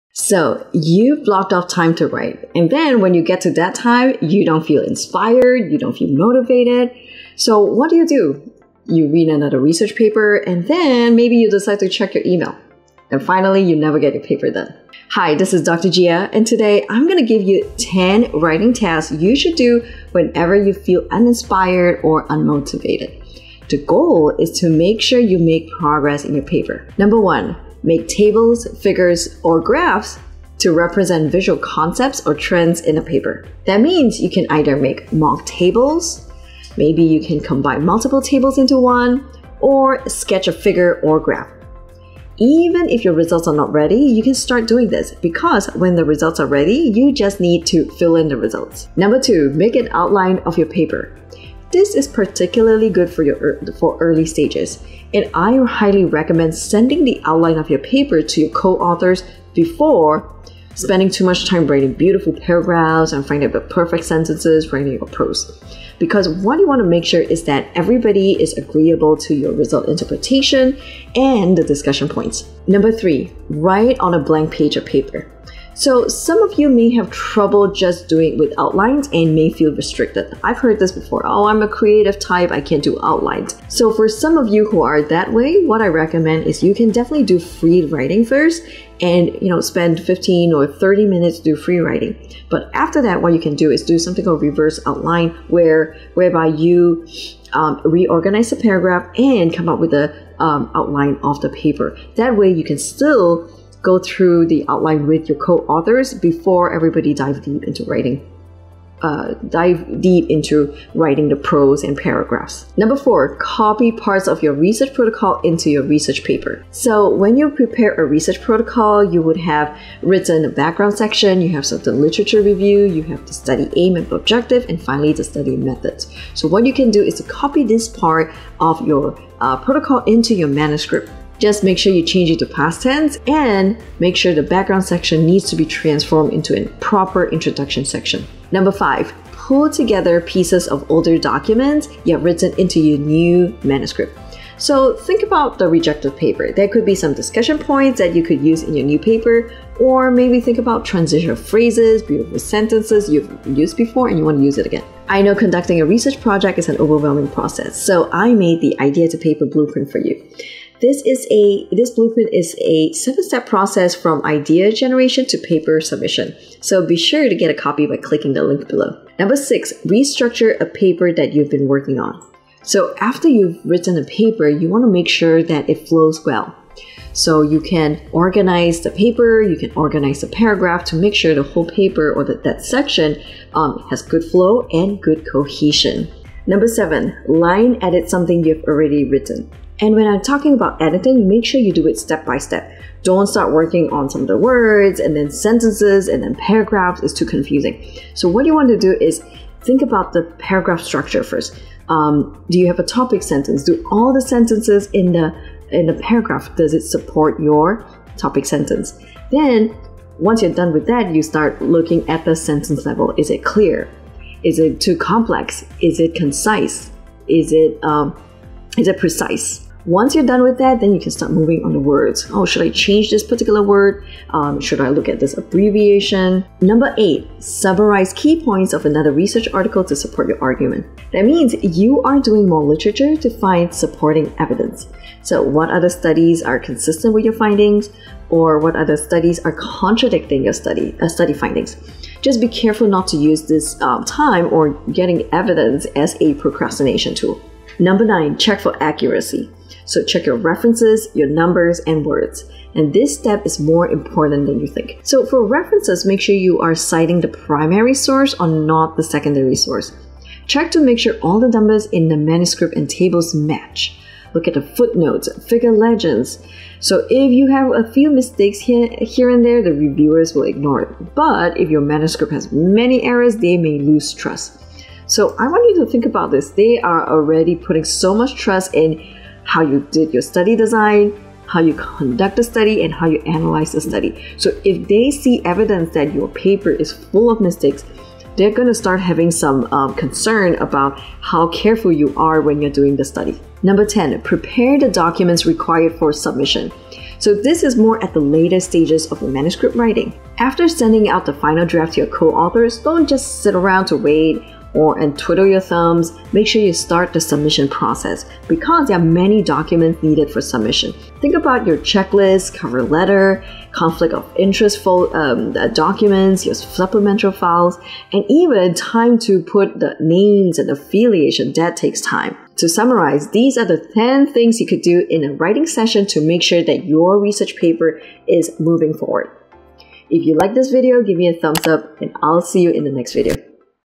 How I Make Progress in Research Paper every day | 10 Things that Count as Writing
Research papers can take a long time to complete.  How to make progress in research paper? You need to schedule time and start writing. What if you get to your writing time but you don't feel motivated? I will share 10 things that count as writing. The goal is to make progress in your paper.

► TIMESTAMPS:
0:00 - Intro
0:32 - Make tables/figures
0:52 - Outline
1:17 - Write on a blank page
1:52 - Copy parts of research protocol
2:19 - Pull together pieces of old writing
2:52 - Restructure
3:07 - Line edit
3:47 - summarize key points from other research
4:11 - check for accuracy
4:58 - Prepare for submission